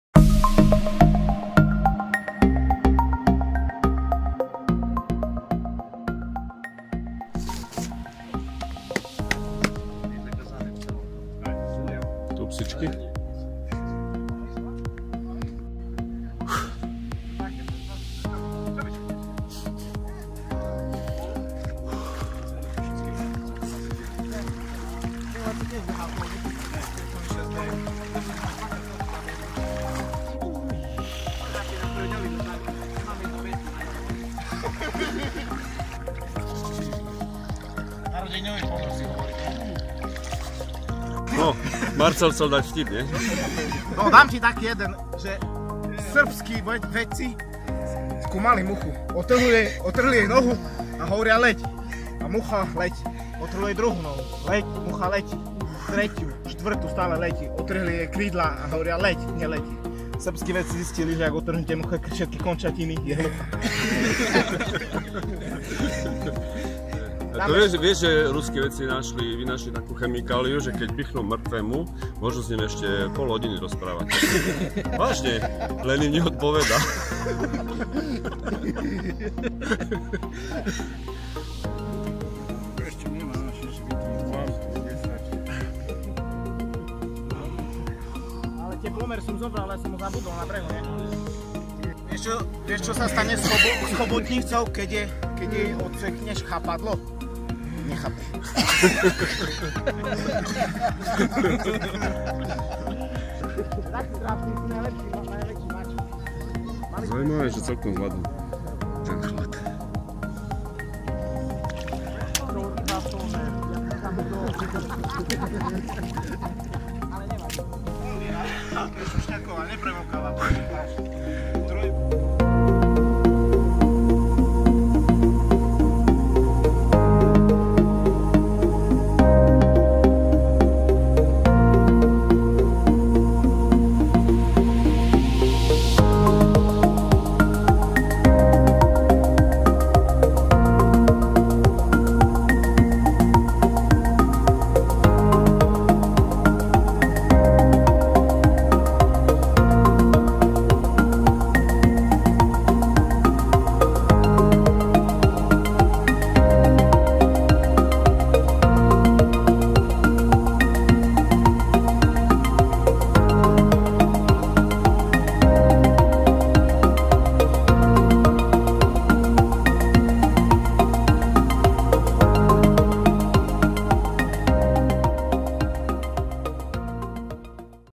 20180311 Ladoborci
Počas tejto zimy som strávil 6 týždňov v oblasti rovníka, no okamžite ako som prišiel domov, tak som šiel otužovať. A potom nemám žiadne problémy ani s domácimi chrípkovými vírusmi a ani s tropickými vírusmi typu Dengue alebo Malaria
Imunitný systém pracuje skvele :)